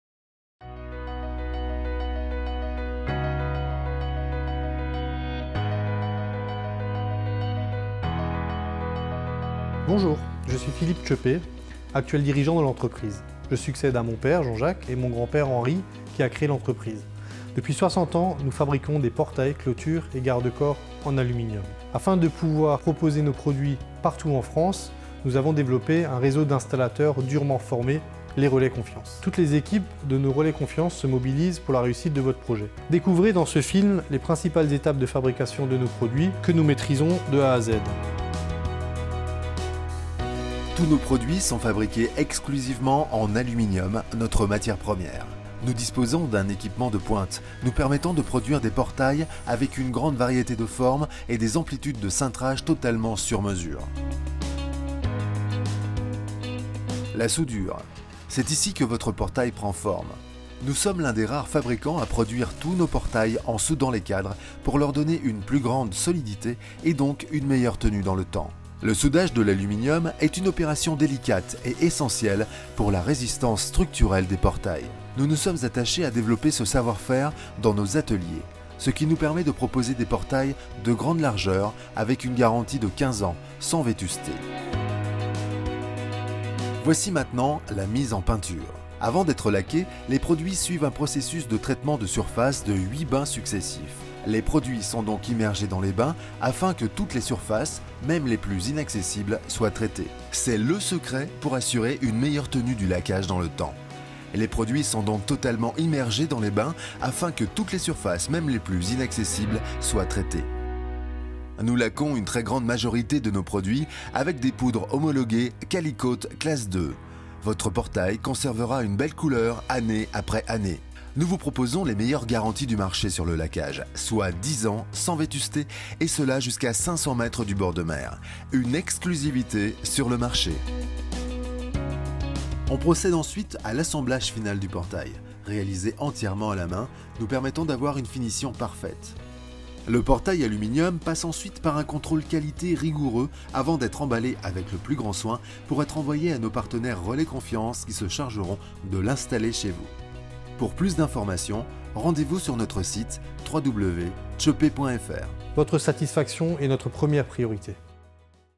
TSCHOEPPE - Présentation d'entreprise - 2018
Découvrez les étapes de fabrication des portails et clôtures aluminium Tschoeppé. Des produits personnalisables, 100% sur-mesure.